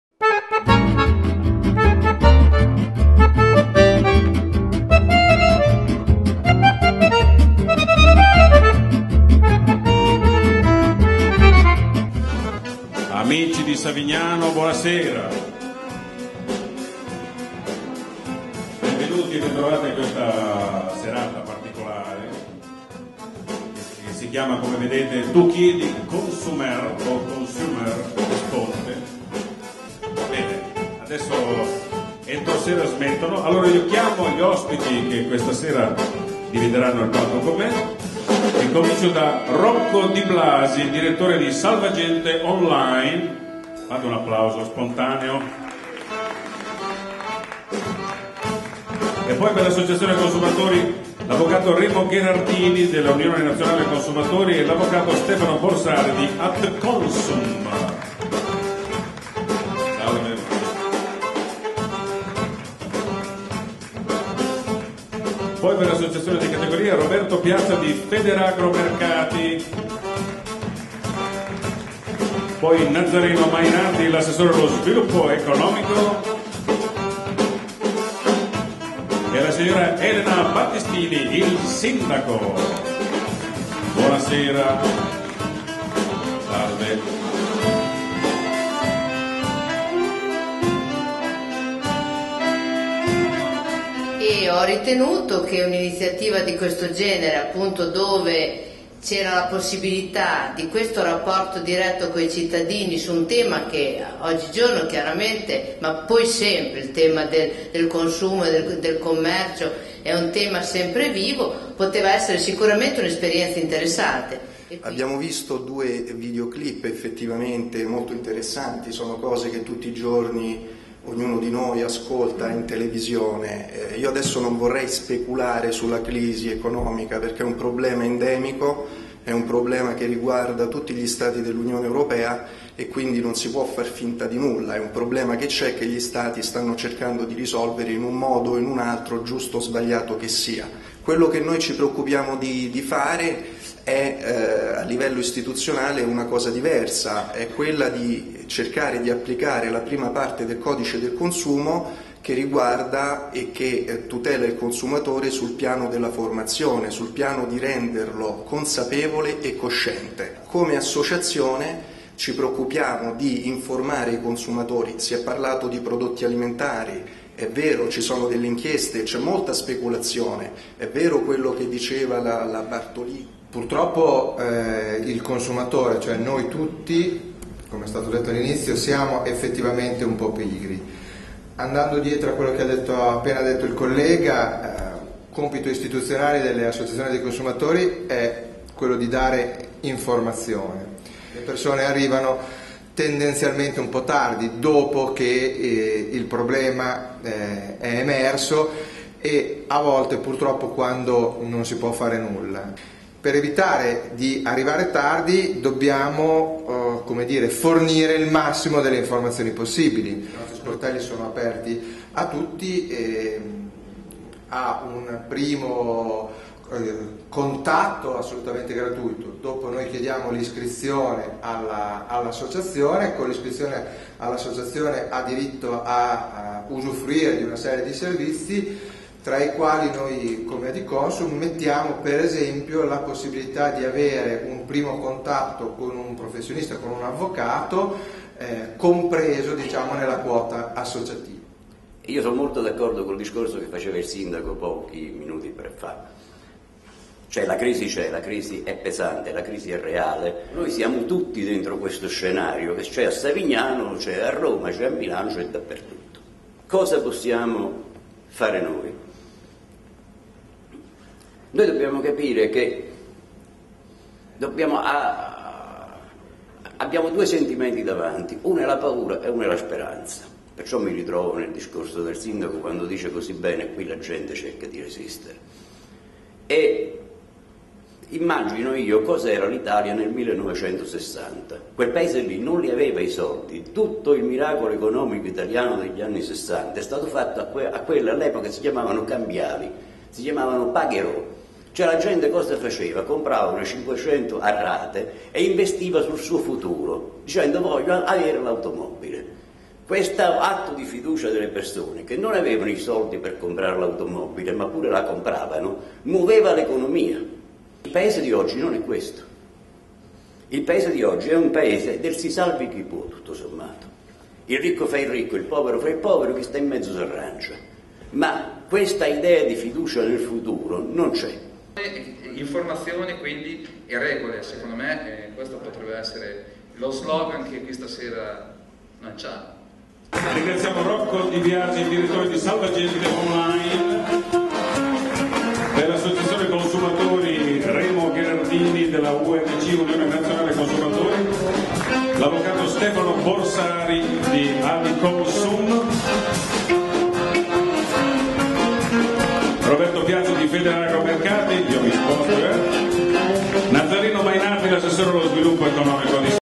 Talk Show Tu chiedi, Consumer risponde a Savignano sul Rubicone (RN) 05/02/2010
Venerdì 5 febbraio presso la Sala Allende il talk show animato dal comico Gianni Fantoni.